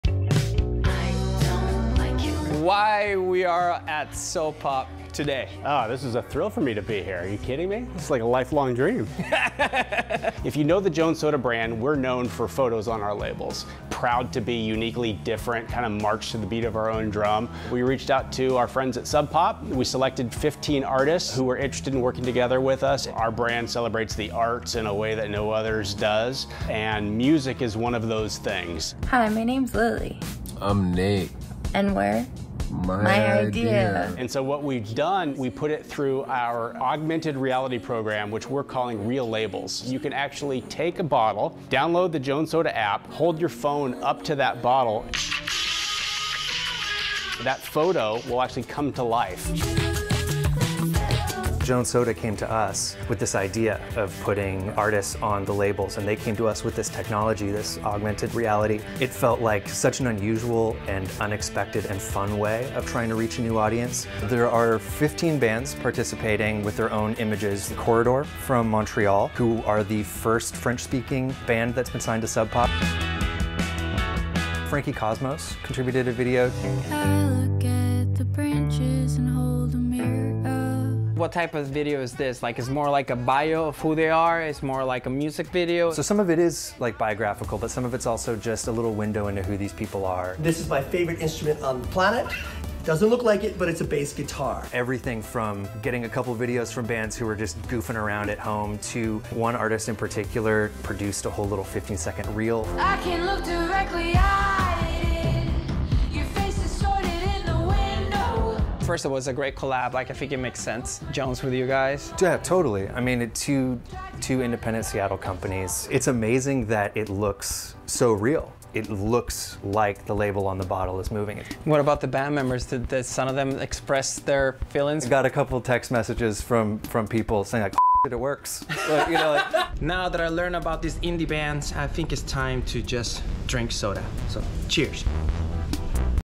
The 'Summer of Indie Music' Jones Soda experience features 15 Sub Pop and Hardly Art artists
Jones Soda Co., known for its unconventional flavors and user-submitted photo labels, has teamed with Sub Pop Records and sister record label Hardly Art to promote 15 artists with albums dropping this summer. This is the latest collaboration between the two iconic Seattle companies both known for over 25 years of artistic innovation through music and imagery.

“If you know the Jones Soda brand we are known for photos on our labels, proud to be uniquely different and march to the beat to your own drum,” said Eric Chastain from Jones Soda.

Through Jones’ REEL LABELS Augmented Reality label series, soda and music fans can see and hear exclusive clips from Sub Pop and Hardly Art artists by scanning soda bottles with the Jones mobile app.

“It's amazing that it looks so real, it looks like the label on the bottle is moving,” said Nick Duncan from Sub Pop Records. “Some of it is like biographical, but some of it is also is just a little window of who these people are.”

After the long pandemic-forced hiatus from the live music experience, Jones sees this as a great opportunity to use its labels to promote up-and-coming bands and celebrate an art form that is so important to the culture of the Jones community.

“Our brand celebrates the art in a way that no other does and music is one of those things,” Eric said.

Indie music enthusiasts can discover new bands as they sip their favorite Jones flavors. Featured artists are based everywhere from Seattle, New York, Chicago, Austin, Virginia, New Jersey, and Massachusetts to Toronto, Montreal, and Tokyo, providing an eclectic mix of sounds to show the diversity of tastes represented by Sub Pop, Hardly Art, and Jones. Independent bands with unique perspectives.

For the “Summer of Indie Music” edition the artist are:

Aeon Station, My Idea, Kiwi Jr., Guerilla Toss, Cartel Madras, Colleen Green, LALA LALA, Hannah Jadagu, Corridor, Bria, Sweeping Promises, Lael Neale, Frankie Cosmos, and Chai.

Maybe you’ll find a new favorite band!